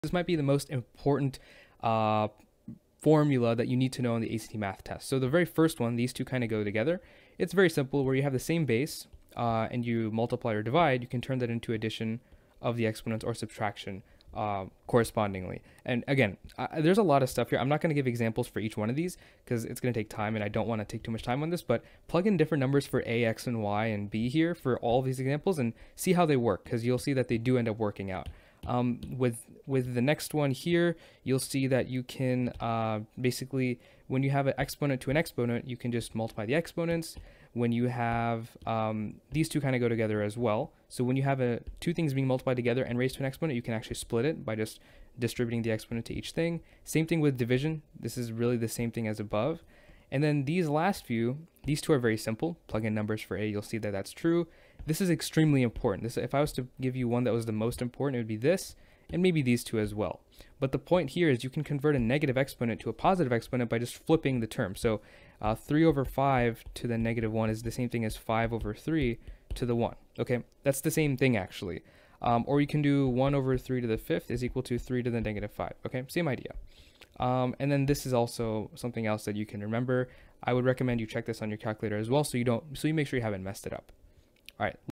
ACT Math: Exponent Properties You MUST Know!
Master ACT Math! We break down exponent properties with simple explanations and examples. Learn essential formulas for the ACT, including how to handle negative exponents and exponent rules. Boost your score now!